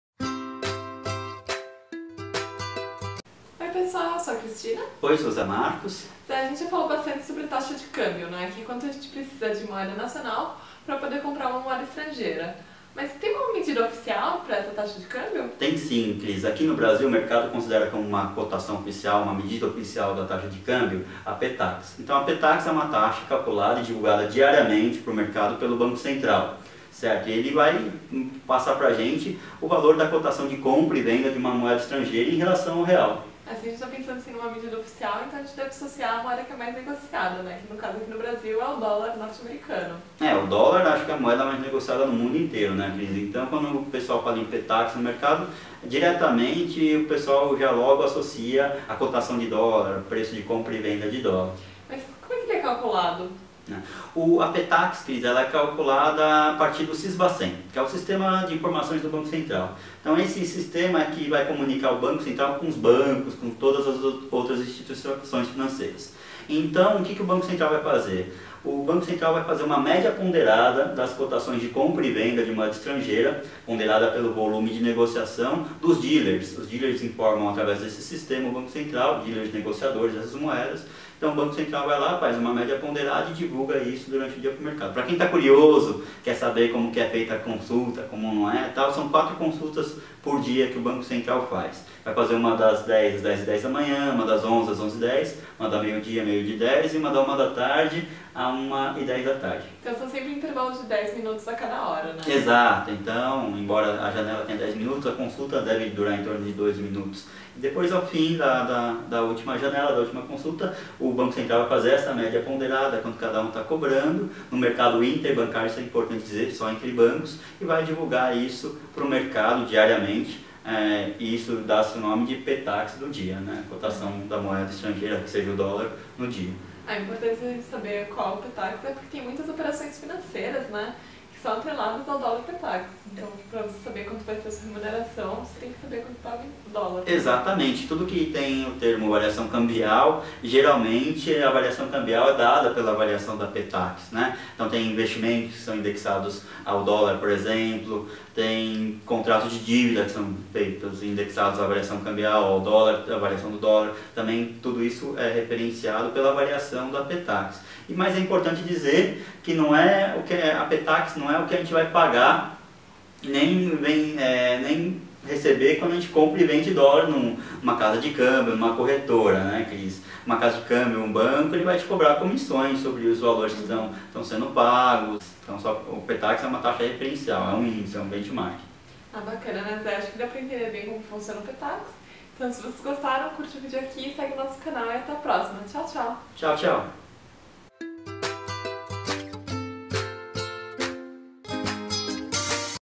Ptax - Taxa de Câmbio
Bate papo sobre a Ptax.

Gostou? Envie sugestões de temas e dúvidas para:
Finanças 101